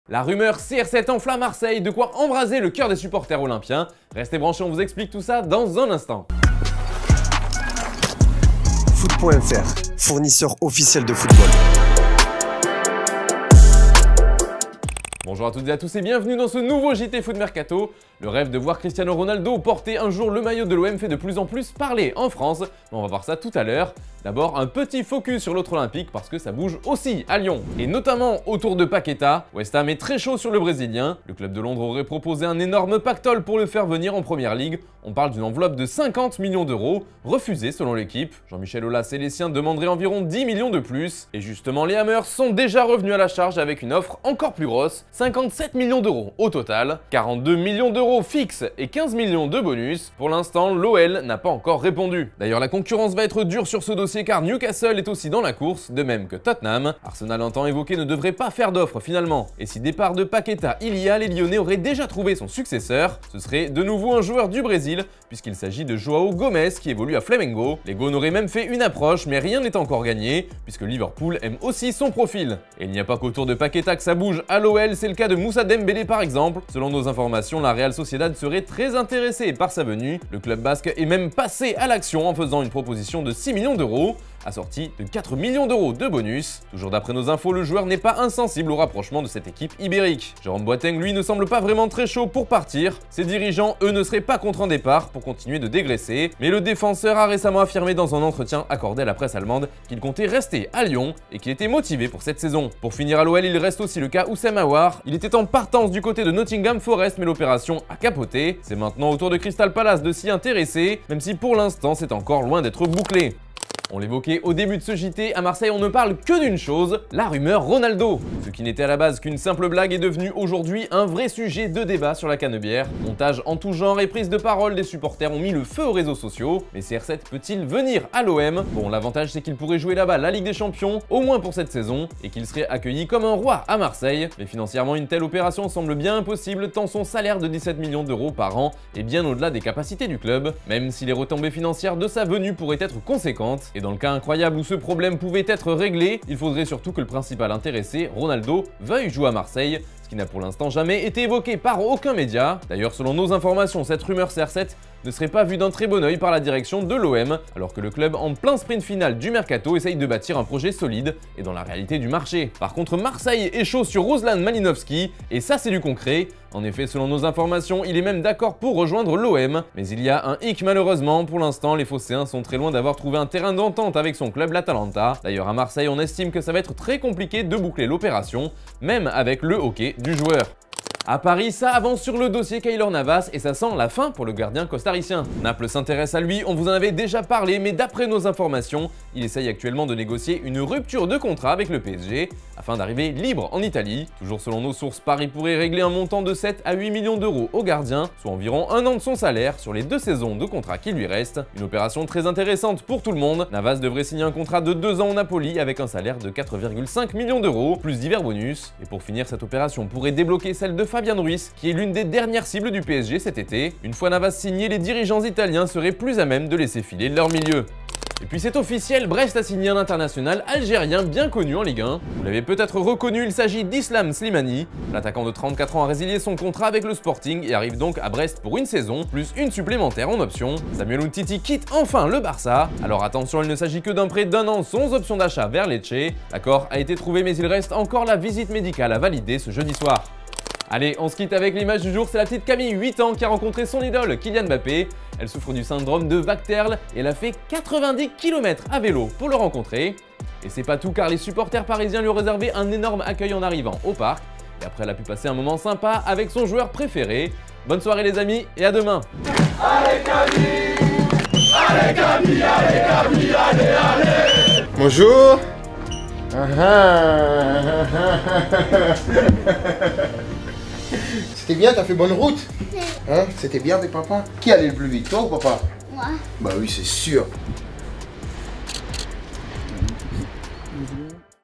La FOLIE Cristiano Ronaldo met le FEU à Marseille | JT Foot Mercato
Au programme de ce JT Foot Mercato : les derniers dossiers brûlants de l'OL, les supporters de l'OM rêvent de Cristiano Ronaldo et les officiels du jour.

00:00 Teaser
00:27 Ça chauffe à l'OL
01:53 Rumeur CR7 à l'OM
03:00 La fin pour Navas au PSG
03:37 Les officiels du jour
04:00 L'image du jour